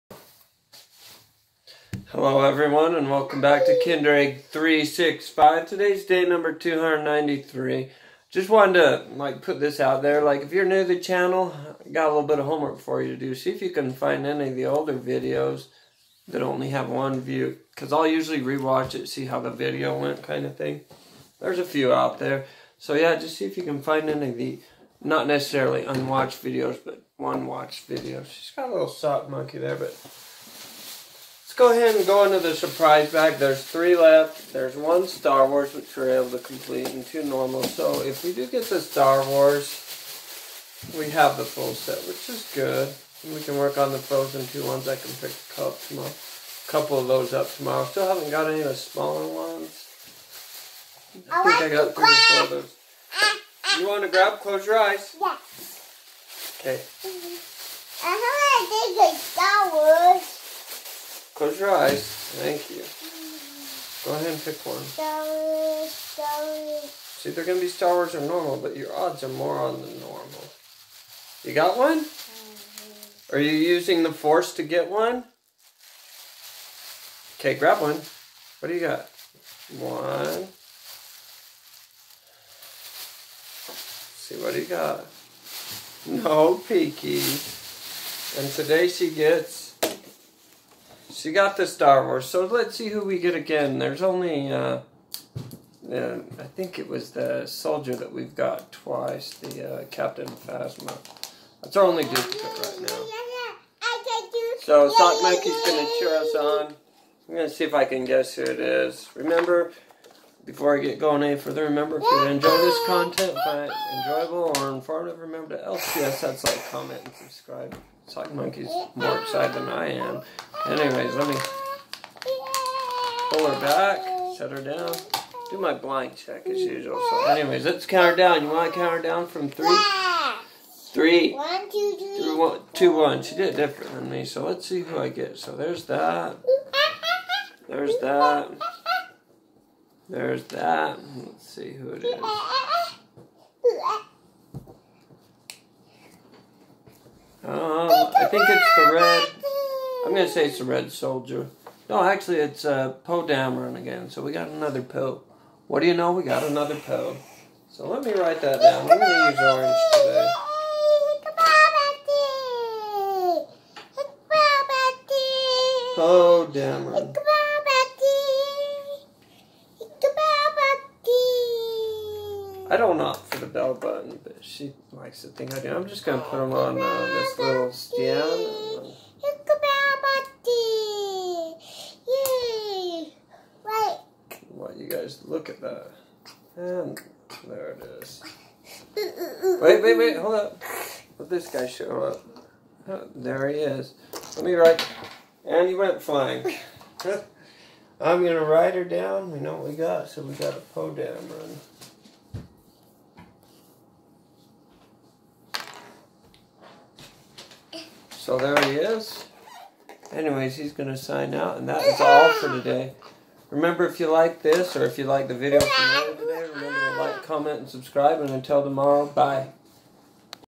Kinder Egg 365 Day #293
Today I talk about how there are solo watched videos, see if you can find them, and then I open up a Kinder Joy from the suprise bag!!!